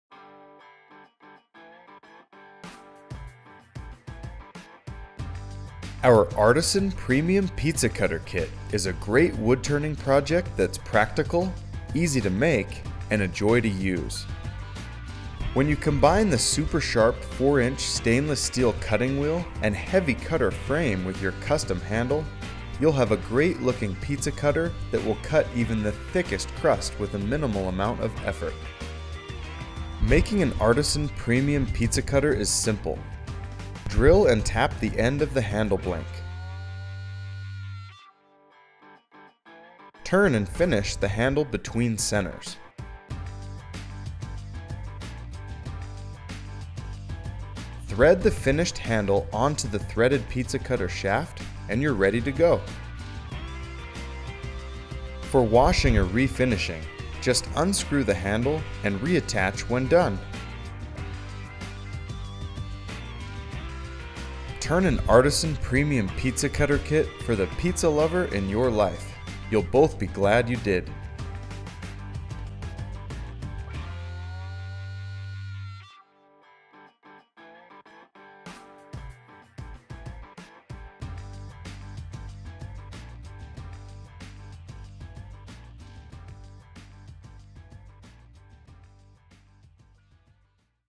Premium Pizza Cutter Kit (Woodturning Project)
The Premium Pizza Cutter Kit is practical, easy to make and a joy to use.

When you combine the super sharp 4" stainless steel cutting wheel and heavy cutter frame with your hand turned cutter handle, you'll find cutting even the thickest crust easy with a minimal amount of effort.

Making an Artisan® Premium Pizza Cutter is simple.  Drill and tap the end of the handle blank, turn and finish the handle between centers, thread the finished handle onto the threaded pizza cutter shaft and you're ready to go.  For washing or refinishing, just unscrew the handle and reattach when done.

•Large 4" stainless steel cutting wheel
•Unique cutter frame design positions weight above the blade
•Cuts faster with less effort
•3 times heavier than regular pizza cutters
•Threaded shaft for easy disassembly
•Dishwasher safe (with handle removed)
•Adds personality and flair to your kitchen
•100% Satisfaction guaranteed

Minimum blank size: 1-1/2" x 1-1/2" x 6" long

- SOCIAL MEDIA -